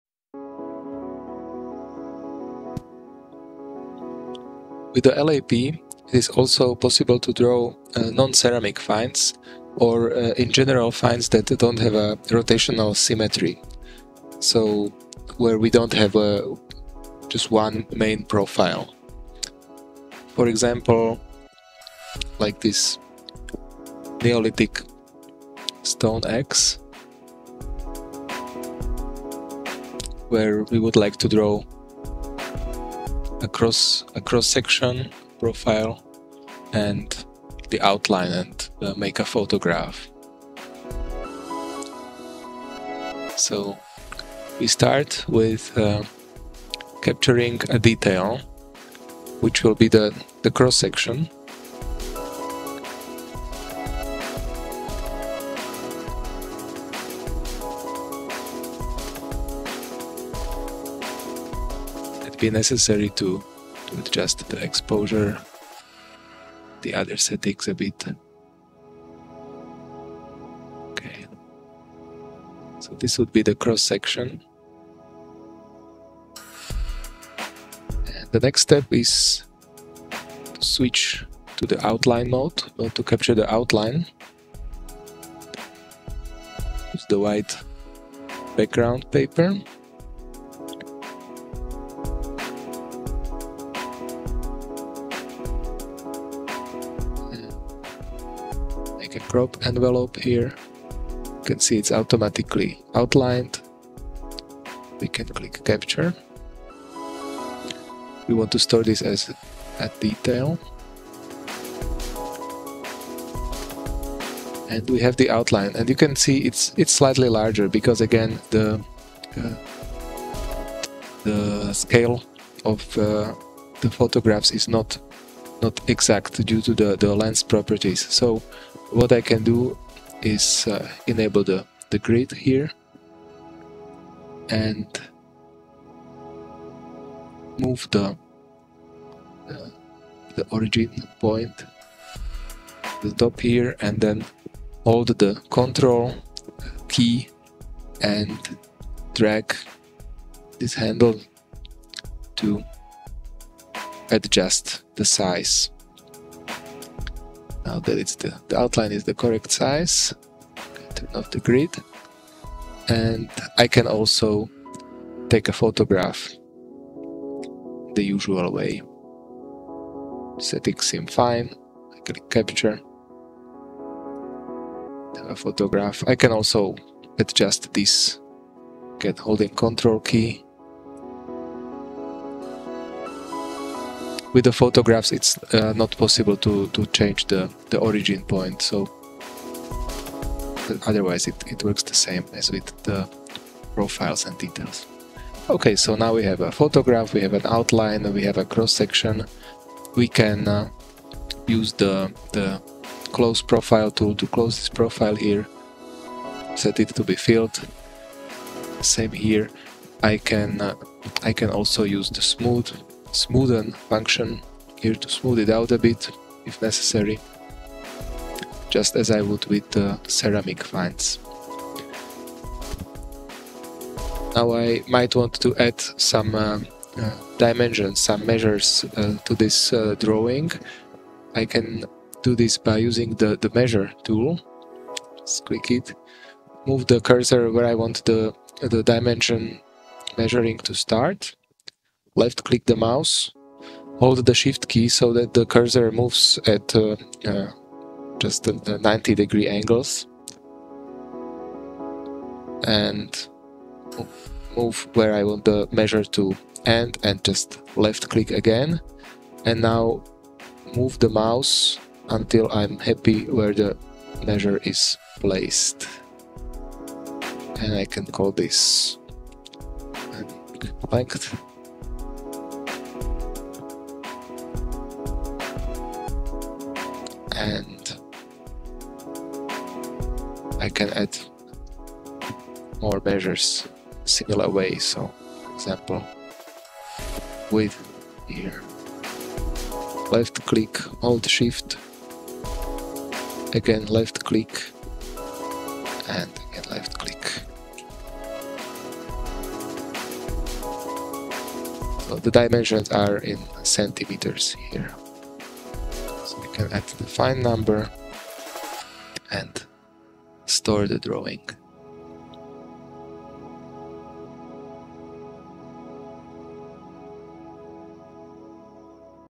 Laser Aided Profiler 4 - Draw: Outline, Non-ceramic Finds
• Drawing Non-ceramic Finds
• Using the Outline Function